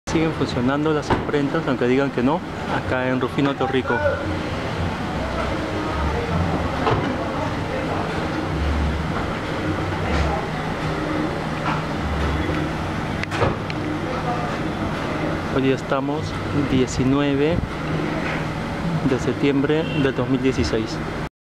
Rufino Torrico - Imprentas trabajando
Aún estas imprentas trabajan a puertas cerradas en una de las tiendas de la casa Barbieri en el lado de la calle de Rufino Torrico, nuevamente se hace denuncia de éste hecho a las autoridades respectivas.  A pesar de haber sido clausuradas hacen caso omiso a la orden de la autoridad.